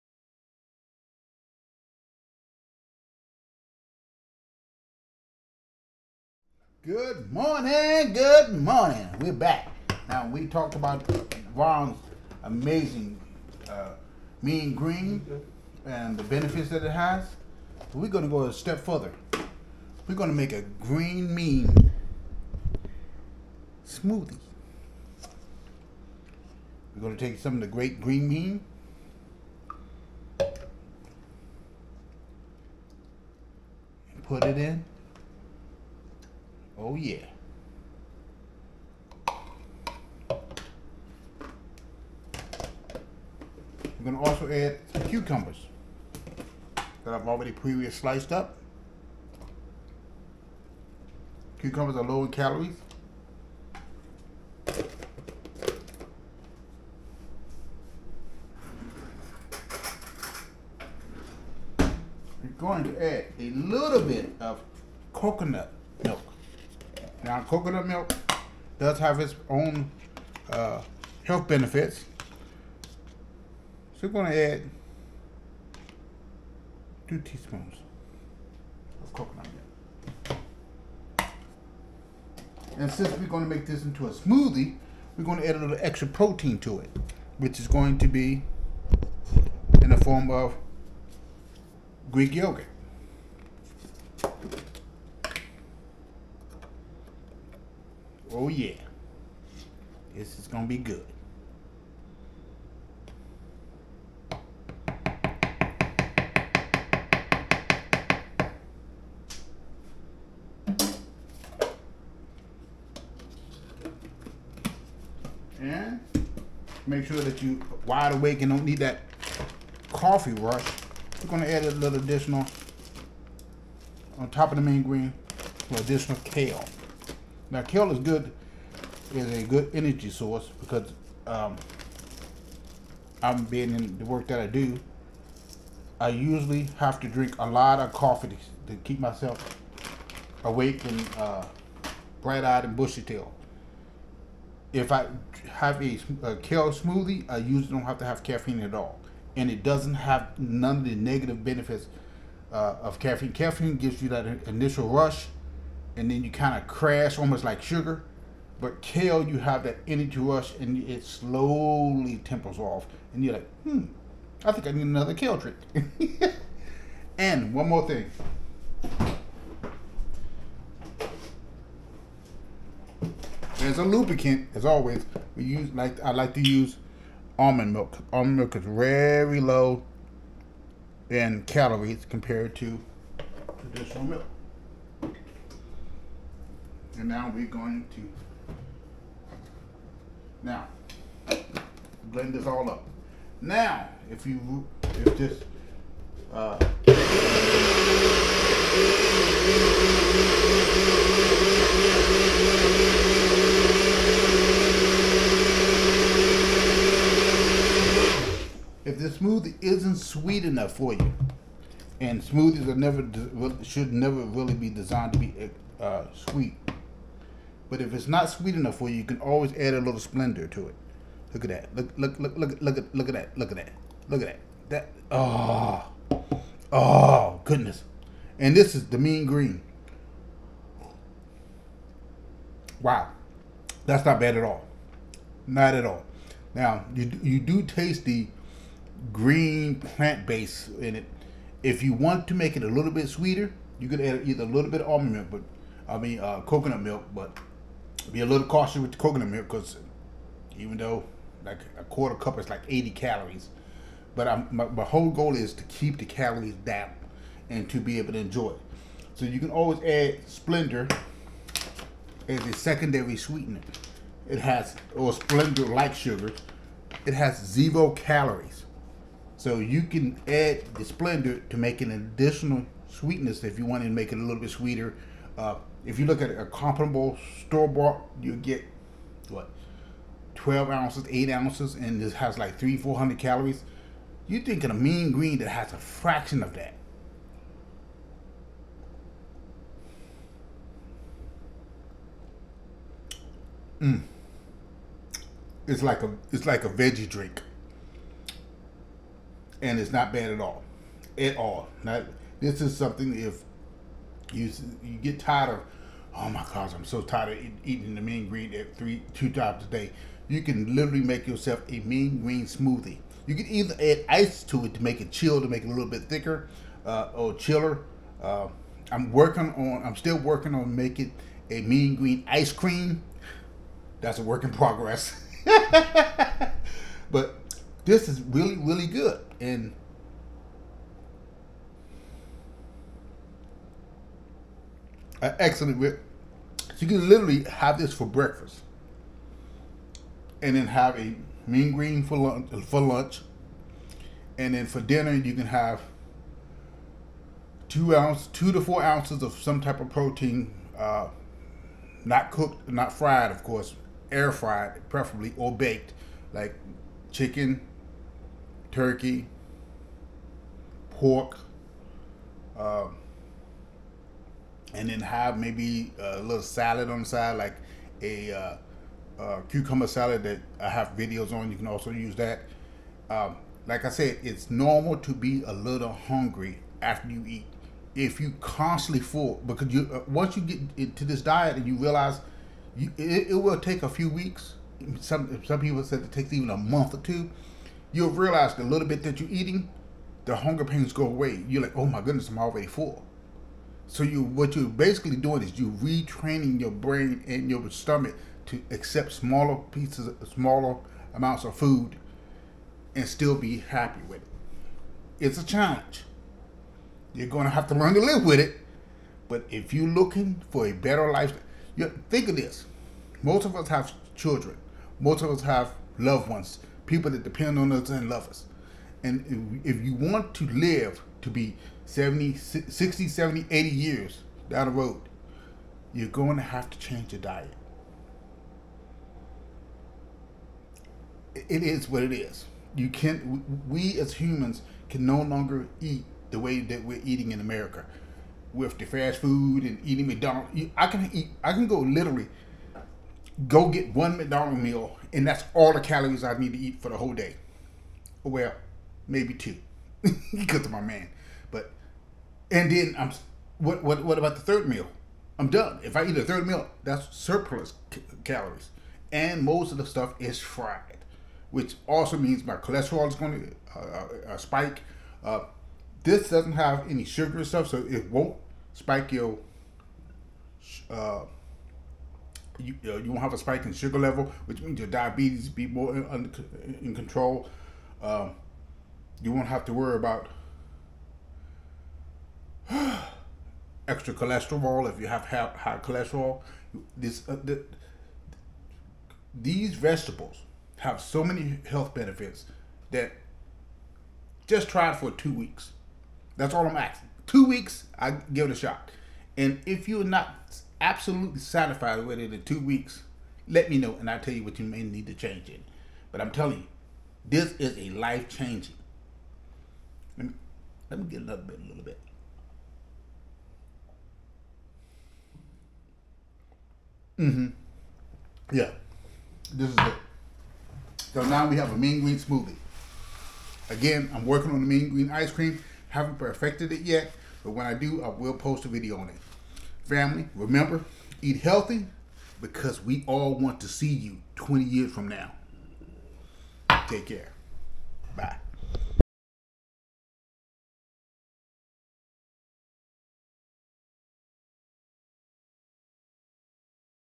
Ron's mean green smoothie
"Mean green" could refer to a variety of things, but it is commonly used to describe a dish or drink that is packed with healthy greens like spinach, kale, or collard greens. These greens are typically blended with other ingredients like fruit, yogurt, or nut milk to create   bowl.